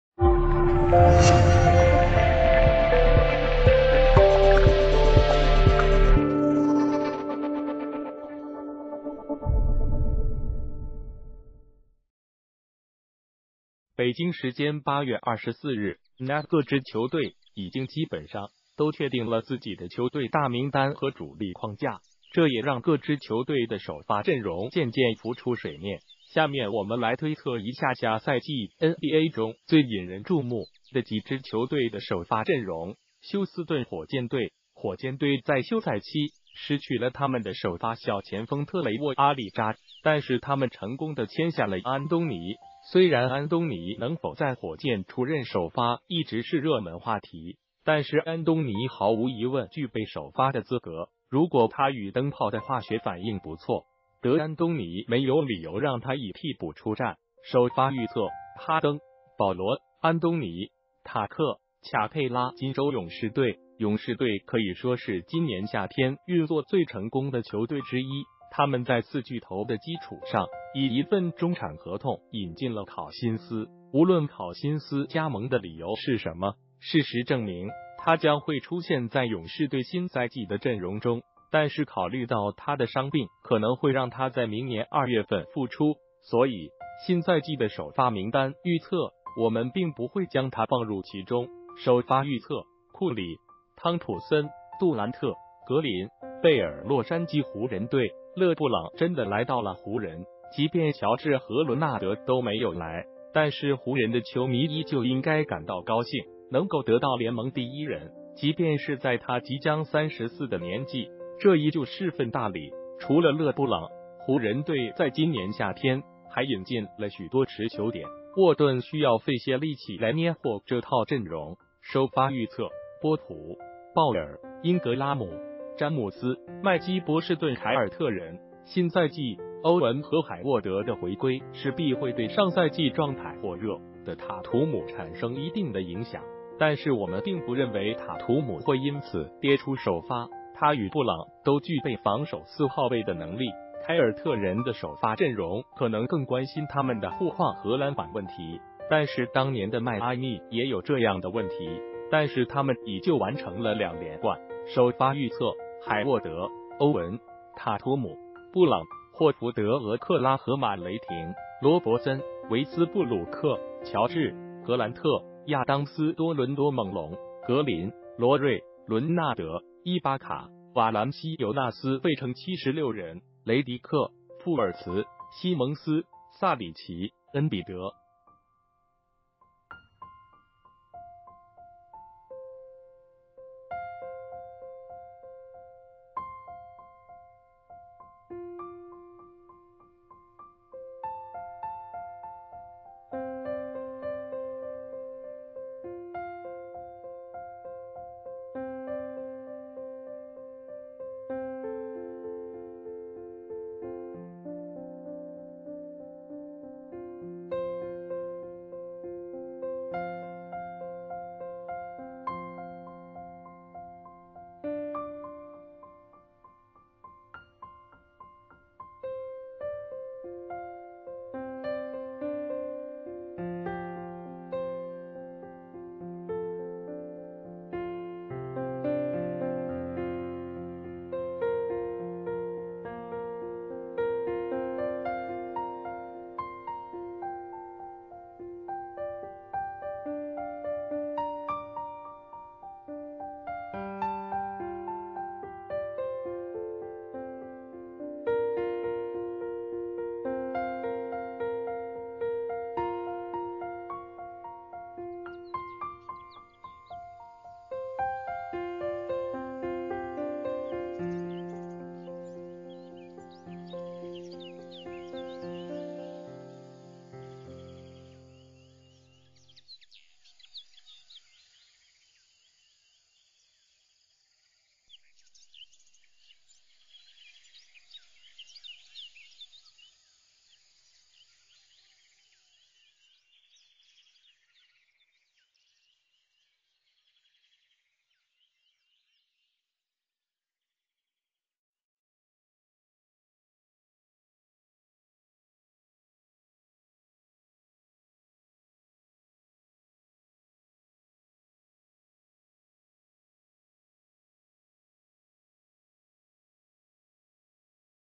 新赛季球队首发名单，湖人最头疼
谢谢！北京时间8月24日，NAB各支球队已经基本上都确定了自己的球队大名单和主力框架，这也让各支球队的首发阵容渐渐浮出水面，下面我们来推测一下下赛季NBA中最引人注目的几支球队的首发阵容。休斯顿火箭队：火箭队在休赛期失去了他们的首发小前锋特雷沃-阿里扎，但是他们成功地签下了安东尼，虽然安东尼能否在火箭出任首发一直是热门话题，但是安东尼毫无疑问具备首发的资格，如果他与灯泡的化学反应不错，德安东尼没有理由让他以替补出战。首发预测：哈登、保罗、安东尼、塔克、卡佩拉金州勇士队：勇士队可以说是今年夏天运作最成功的球队之一，他们在四巨头的基础上，以一份中产合同引进了考辛斯，无论考辛斯加盟的理由是什么，事实证明，他将会出现在勇士队新赛季的阵容中。但是考虑到他的伤病可能会让他在明年二月份复出，所以，新赛季的首发名单预测我们并不会将他放入其中。首发预测：库里、汤普森、杜兰特、格林、贝尔洛杉矶湖人队：勒布朗真的来到了湖人，即便乔治和伦纳德都没有来，但是湖人的球迷依旧应该感到高兴，能够得到联盟第一人，即便是在他即将34的年纪，这依旧是份大礼。除了勒布朗，湖人队在今年夏天还引进了许多持球点，沃顿需要费些力气来捏和这套阵容。首发预测：波普、鲍尔、英格拉姆、詹姆斯、麦基波士顿凯尔特人：新赛季，欧文和海沃德的回归势必会对上赛季状态火热的塔图姆产生一定的影响，但是我们并不认为塔图姆会因此跌出首发，他与布朗都具备防守四号位的能力。凯尔特人的首发阵容可能更关心他们的护框和篮板问题，但是当年的迈阿密也有这样的问题，但是他们以就完成了两连冠。首发预测：海沃德、欧文、塔图姆、布朗、霍福德俄克拉荷马雷霆：罗伯森、维斯布鲁克、乔治、格兰特、亚当斯多伦多猛龙：格林、罗瑞、伦纳德、伊巴卡、瓦兰西尤纳斯费城76人：雷迪克、富尔茨、西蒙斯、萨里奇、恩比德